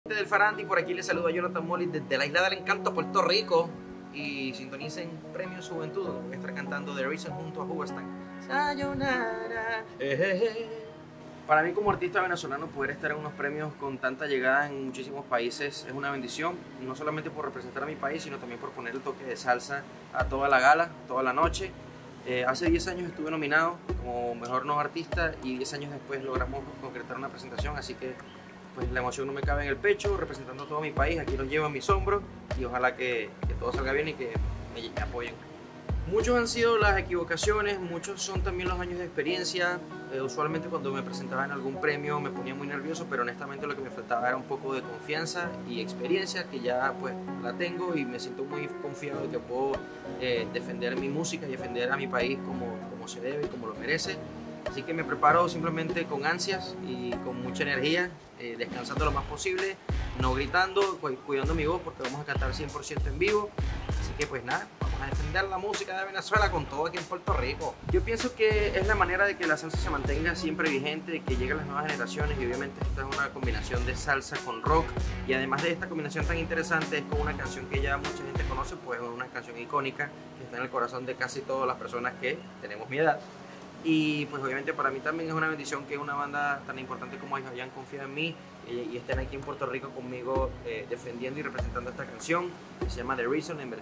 Jonathan Moly saluda al Despelote desde Puerto Rico antes de los Premios Juventud
La prestigiosa gala de los Premios Juventud se verá enriquecida con la presencia estelar de Jonathan Moly, el talentoso cantante venezolano. Su participación en este destacado evento promete ser una oportunidad inigualable para proyectar su carrera a nuevas alturas y cautivar a audiencias de todo el mundo con su voz única y carismática

No te quedes fuera y forma parte YA de El Chismógrafo

¡Somos El Farandi! Dando de qué hablar